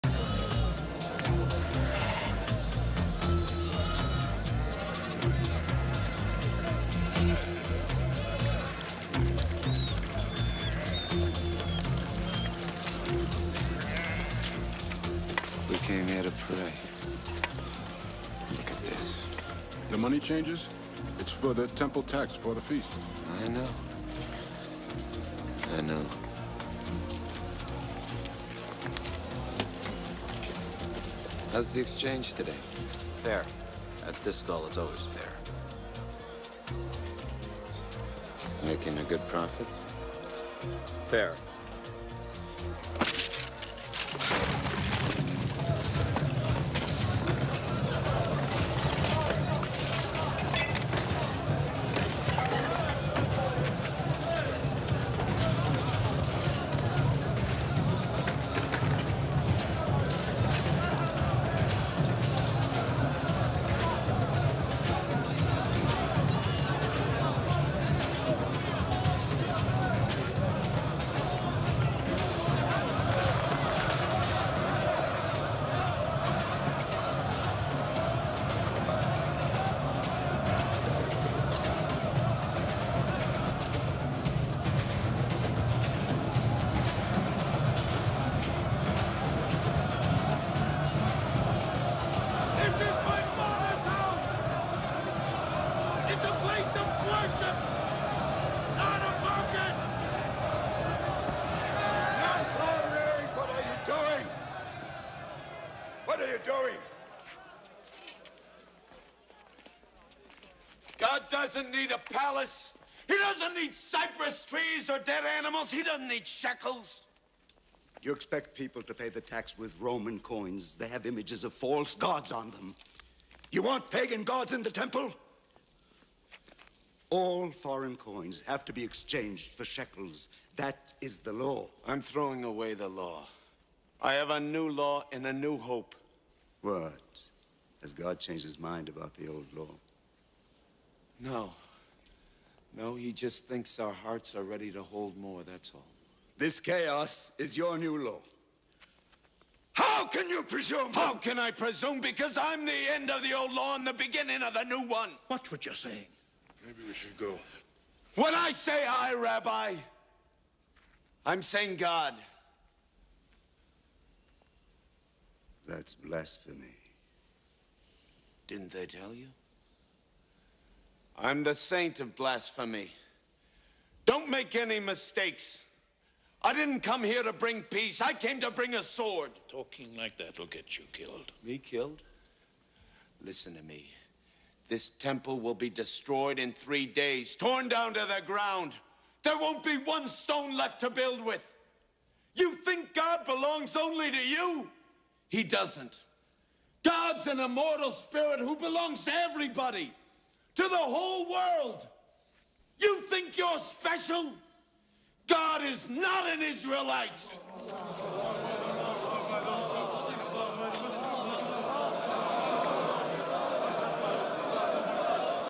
Jesus Christ at the market
The only time Jesus got upset. The same mercantile mentality is still present in some parts of our world.